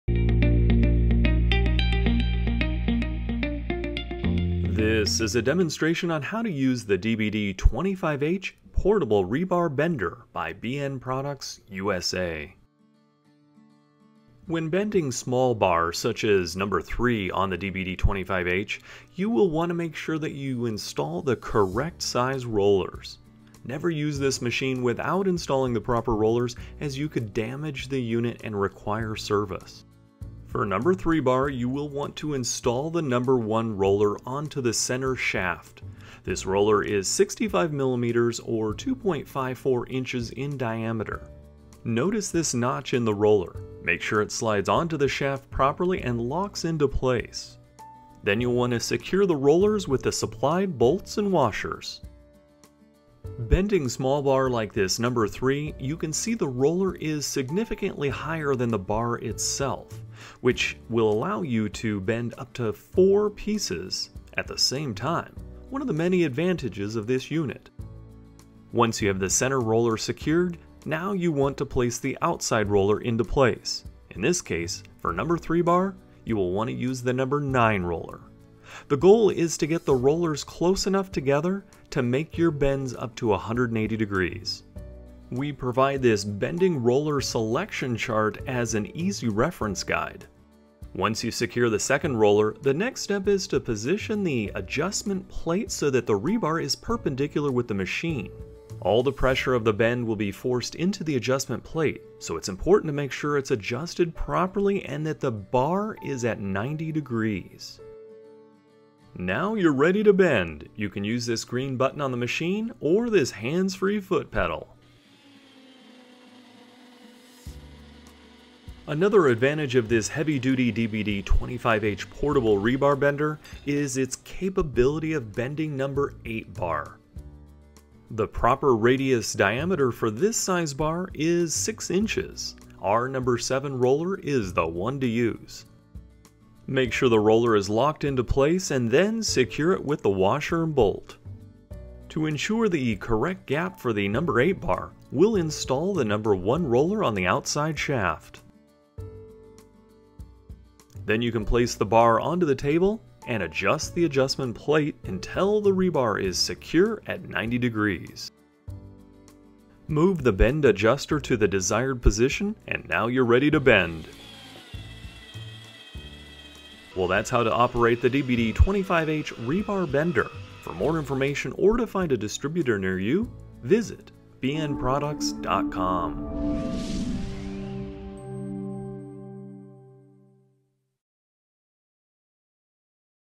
DBD-25H | How to operate the DBD-25H Portable Rebar Bender
This heavy-duty portable bender helps to save valuable man-hours by effortlessly bending rebar up to #8 (1") Grade 60 at job sites or in your shop. This strong electrical-mechanical bender has two preset angle locks for repeating the same angle bend. The lever-type angle selector can be set to accurately bend any angle from 0-180 degrees. Bends (4) each #3 rebar, (3) each #4, (2) each #5 or #6 rebar and (1) #7 or #8 rebar at one time. The roller chart makes it easy to select the correct combination of rollers to achieve an inside bending diameter of six times the rebar diameter. An emergency stop switch and two lifting handles are standard features. The DBD-25H portable rebar bender comes complete with (9) bending rollers in two steel carrying cases, a tool kit, a bender cover, and a standard foot pedal control switch for hands-free bending. Optional circular pattern bending rollers are available. Benner-Nawman backs it with a one-year warranty.